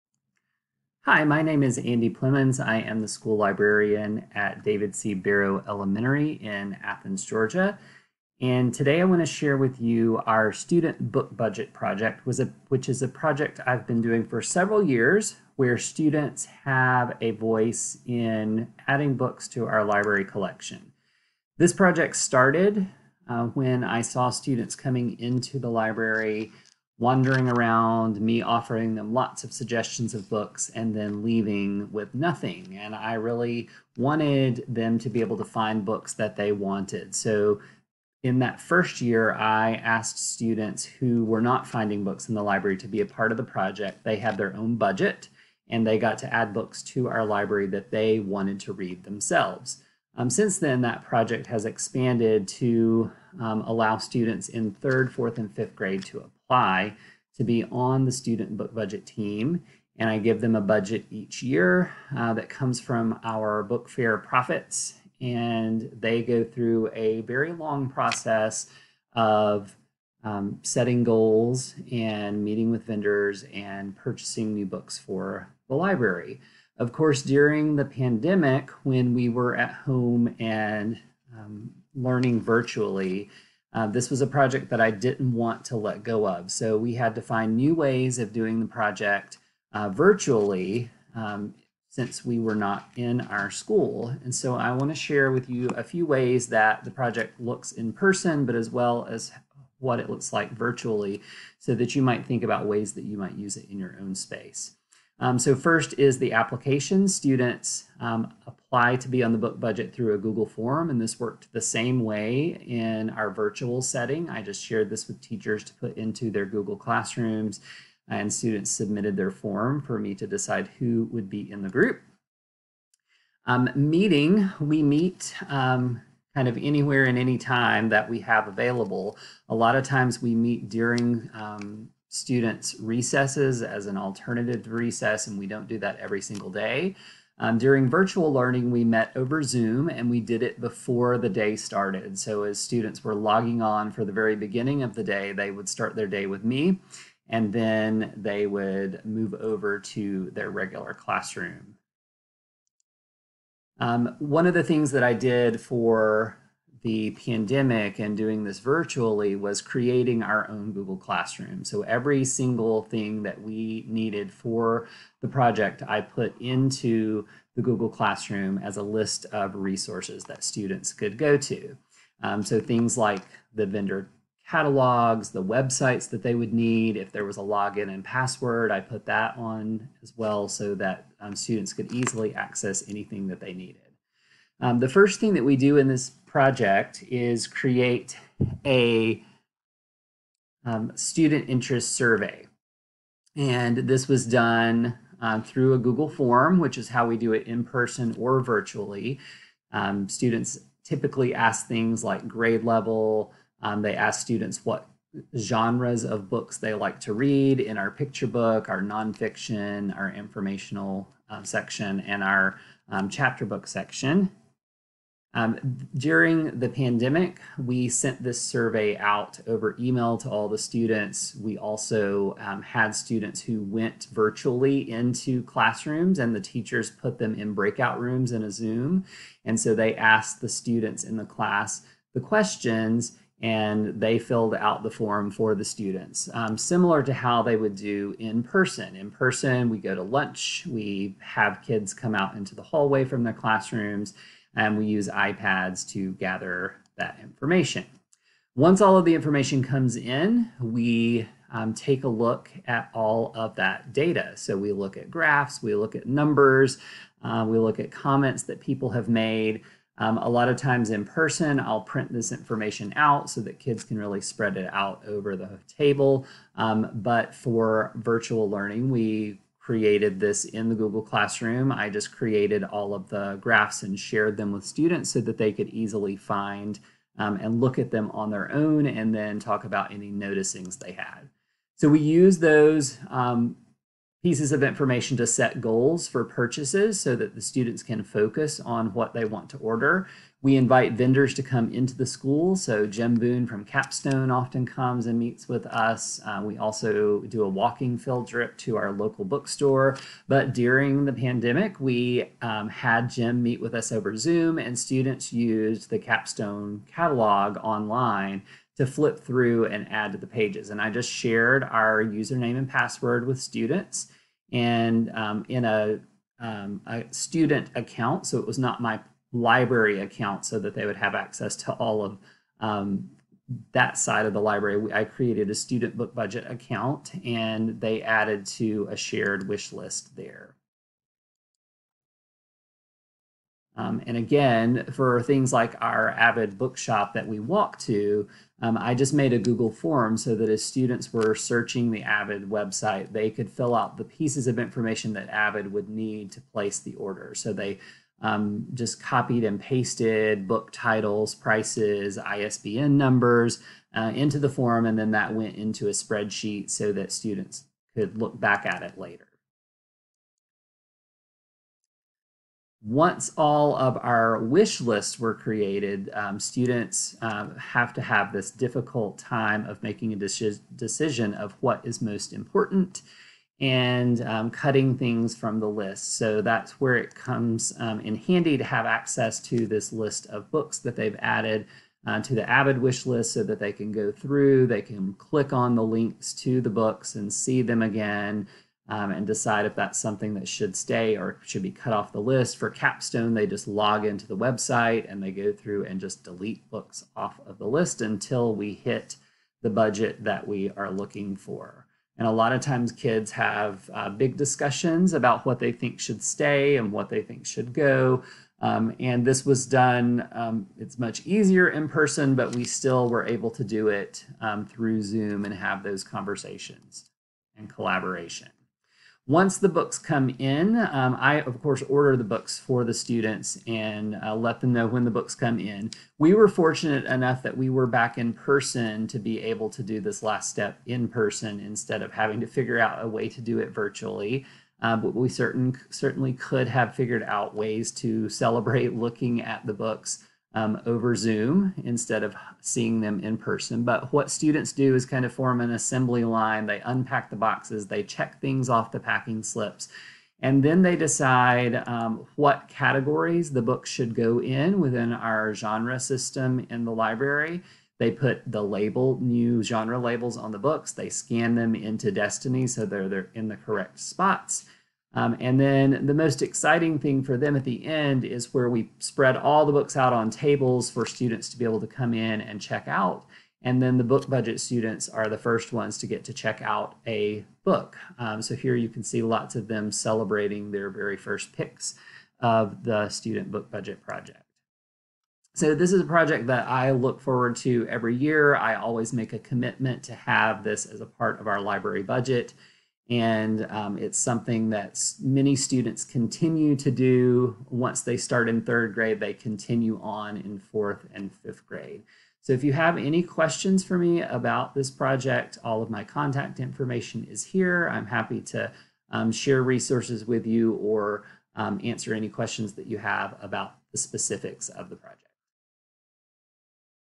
ISTE Student Collection Development
ISTE Librarians Playground presentation 6/28/22
New Orleans

Briefly explains the student book budget project and how it was modified to be virtual during the pandemic distance learning.